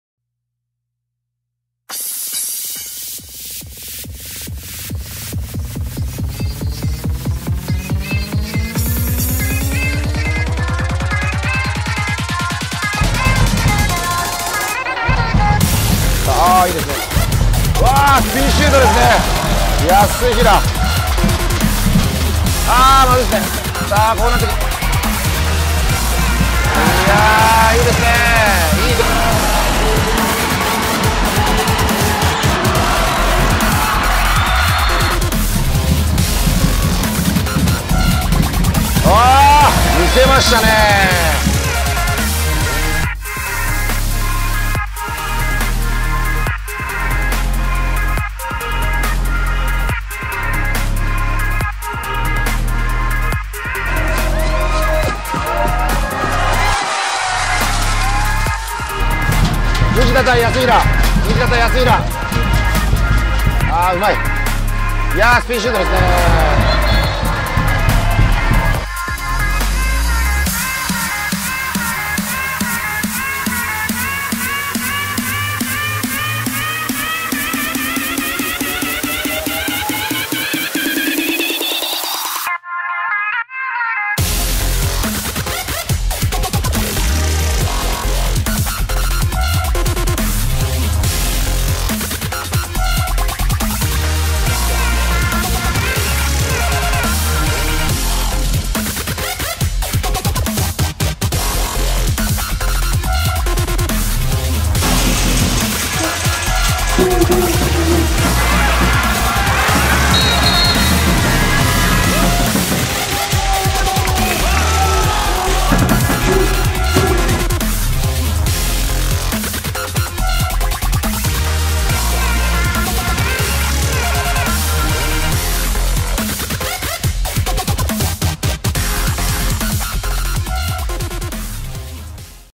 【ハンドボール】高校生のトリッキーなシュートがガチで〇〇だった【高校生】
【Handball】日本の高校総体で活躍したユーススタープレイヤーたちのトリッキーなシュートをまとめました！特にサイドシュートのスピンや速攻におけるキーパーとの一対一、試合の終盤で咄嗟にでるスーパーシュートなど、興奮すること間違いなしのスーパープレイです！ガチでプロの選手にも見たことないくらいの半端ないシュートの数々で圧倒されますw遊びすぎててその後監督に怒られないか心配になるレベルのシュートもちらほらw余談ですが、個人的には38秒あたりの氷見の絶対的存在 安平さんのインハイ決勝残り３分の決死の攻めで見せた、倒れこみながら打つステップシュートが感動的で一番好きです！続きは動画をご覧下さい！

◆オススメ動画や関連動画